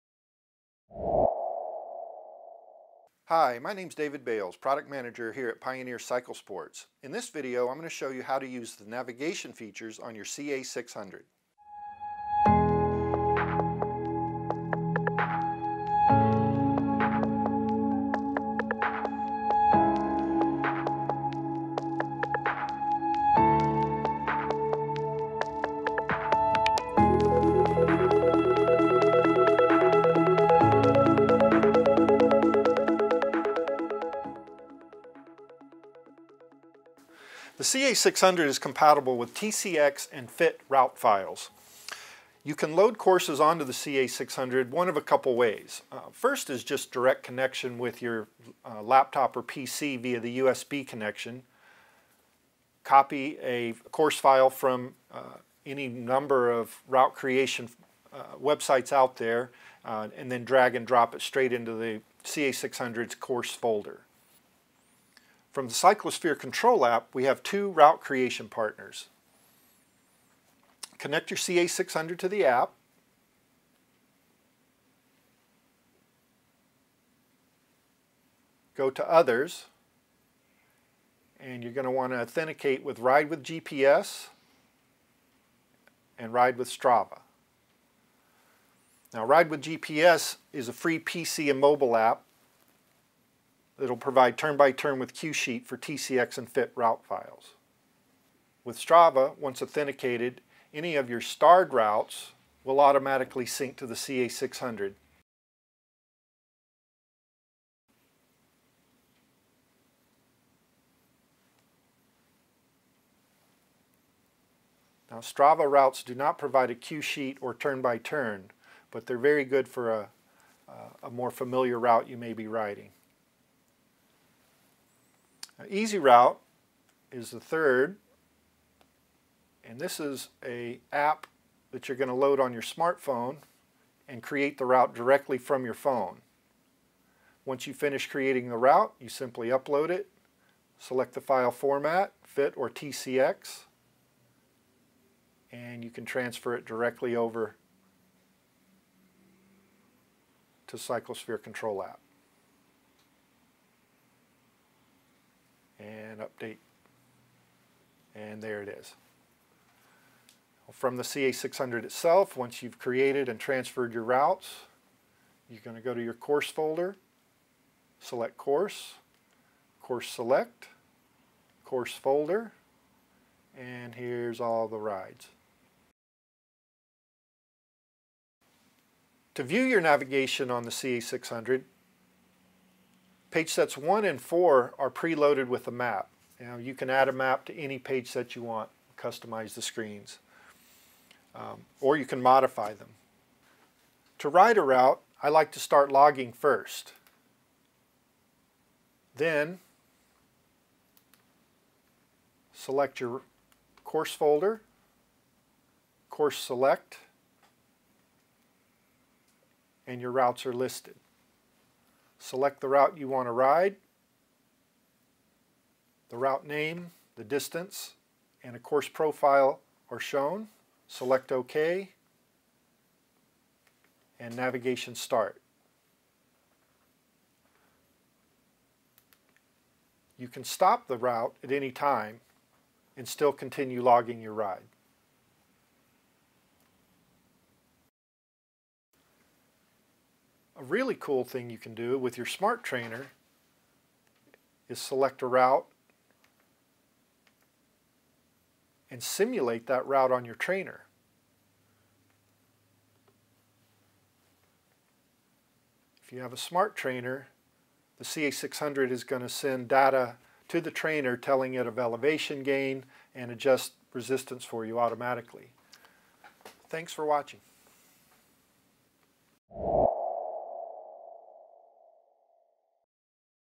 How to - Use the CA600 Cycle Computer for Navigation
A walk through of adding a route to your CA600 cycle computer from one of our route partners, then starting the GPS navigation for a ride or for use with a smart trainer.